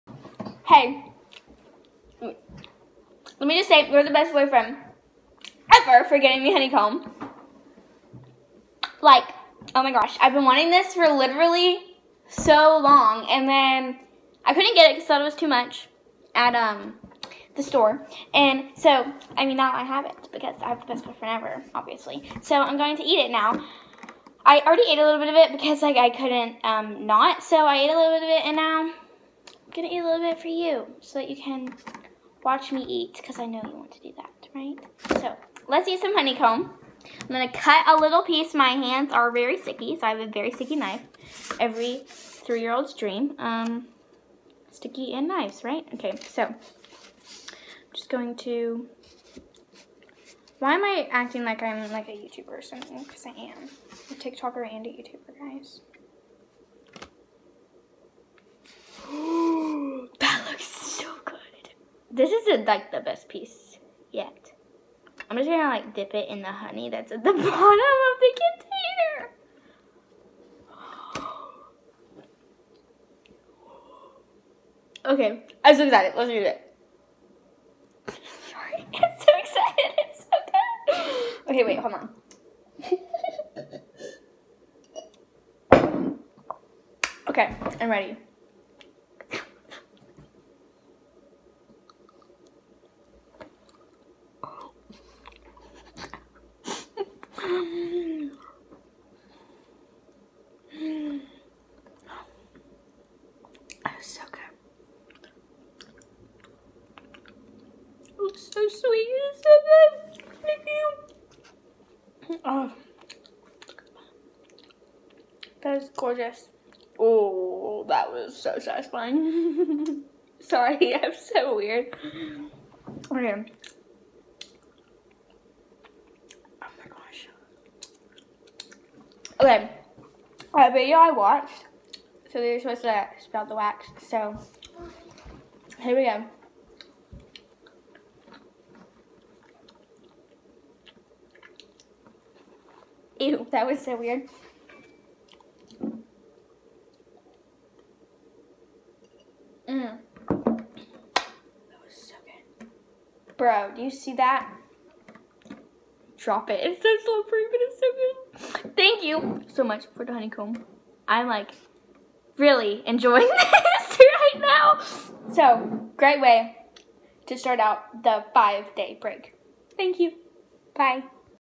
Honeycomb
this is my first time ever trying honeycomb. it was amazing!!!